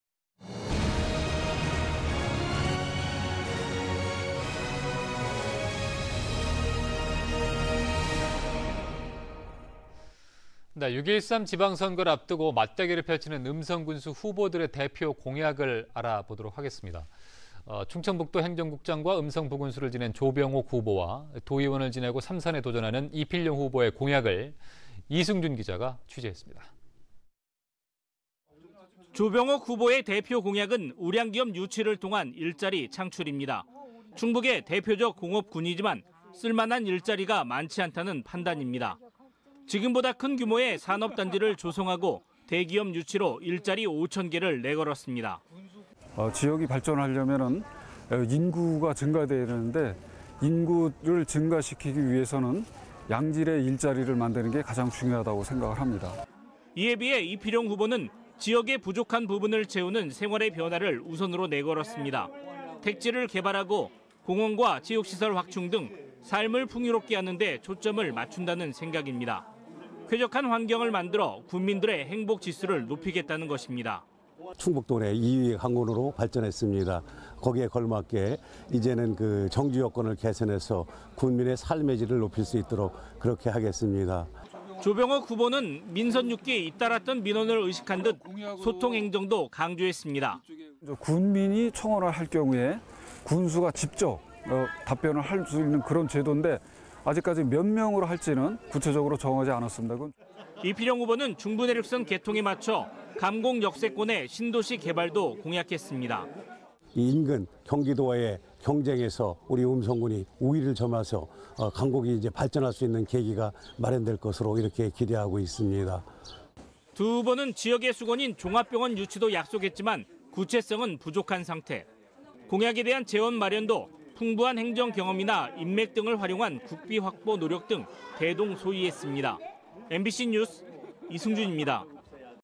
대표공약ㅣ음성군수 후보 공약 비교ㅣMBC충북NEWS
[앵커]
 이번에는 6.13 지방선거를 앞두고
맞대결을 펼치는 음성군수 후보들의
대표 공약을 알아보겠습니다.

 충청북도 행정국장과 음성부군수를 지낸
조병옥 후보와 ,
도의원을 지내고 3선에 도전하는
이필용 후보의 공약을
이승준 기자가 보도합니다.

[기자]
 조병옥 후보의 대표 공약은
우량 기업 유치를 통한 일자리 창출입니다.

 충북의 대표적 공업 군이지만
쓸 만 한 일자리를 많지 않다는 판단입니다.

 지금보다 큰 규모의 산업단지를 조성하고
대기업 유치로 일자리 5천 개를 내걸었습니다.

[ 조병옥 민주당 음성군수 후보]
"지역이 발전하려면 인구가 증가돼야 되는데
인구를 증가시키기 위해서는 양질의 일자리를
만드는 게 가장 중요하다고 생각을 합니다."

 이에 비해 이필용 후보는
지역의 부족한 부분을 채우는 생활의 변화를
우선으로 내걸었습니다.

 택지를 개발하고 공원과 체육시설 확충 등
삶을 풍요롭게 하는 데 초점을 맞춘다는
생각입니다.

 쾌적한 환경을 만들어
군민들의 행복 지수를 높이겠다는 것입니다.

[ 이필용 한국당 음성군수 후보]
"충북 도내 2위의 강군으로 발전했습니다.
거기에 걸맞게 이제는 정주 여건을 개선해서
군민의 삶의 질을 높일 수 있도록
그렇게 하겠습니다."

 조병옥 후보는 민선 6기 잇따랐던 민원을
의식한 듯 소통 행정도 강조했습니다.

"군민이 청원을 할 경우에 군수가
직접 답변을 할 수 있는 제도인데,
아직까지 몇 명으로 할지는
구체적으로 정하지 않았습니다."

 이필용 후보는 중부내륙철도 개통에 맞춰
감곡 역세권에 신도시 개발도 공약했습니다.

"인근 경기도와의 경쟁에서 우리 음성군이
우위를 점해서 감곡이 발전할 수 있는 계기가 마련될 것으로 기대하고 있습니다."

 두 후보는 지역의 숙원인 종합병원 유치도
약속했지만, 구체성은 부족한 상태.

 공약에 대한 재원 마련도
풍부한 행정 경험이나 인맥 등을 활용한
국비 확보 노력 등 대동소이했습니다.
MBC 뉴스 이승준입니다.
(영상취재 허성대)